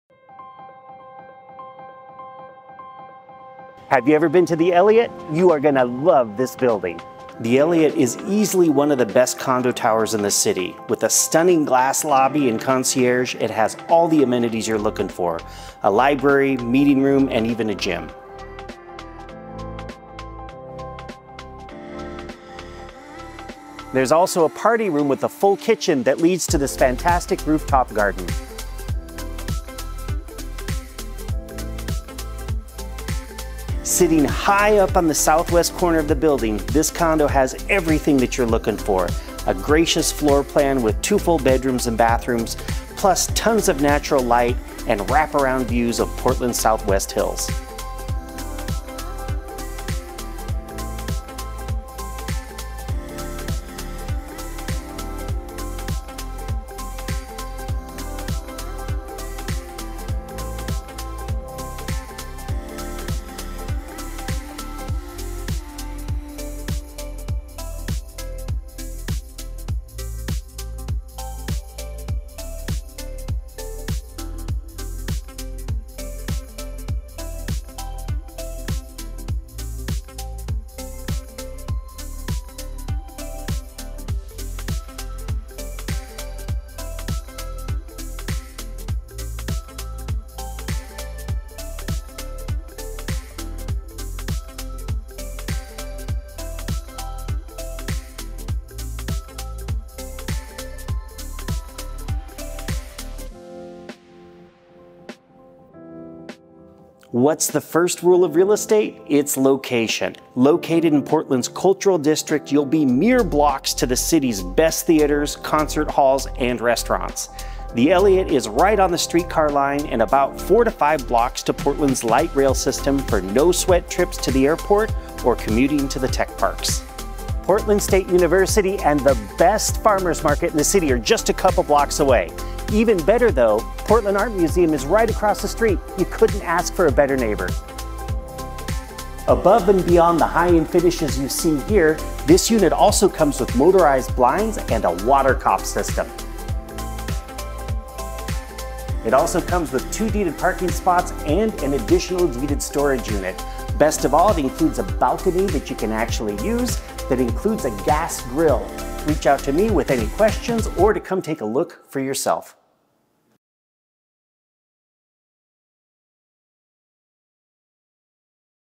Modern Condo with Amazing Views in Portland! ~ Video of 1221 SW 10th Ave. #1306 ~ Downtown Portland
SW Corner location at The Eliot with Sweeping City Views! Abundant Natural Light, automatic blinds, balcony with gas grill. 2 bedrooms/2 bathrooms - fitness facility, meeting room, party room, outdoor garden/barbeque area. Fabulous location. Contact Ron Milligan with RE/MAX Select for more info or your personal tour.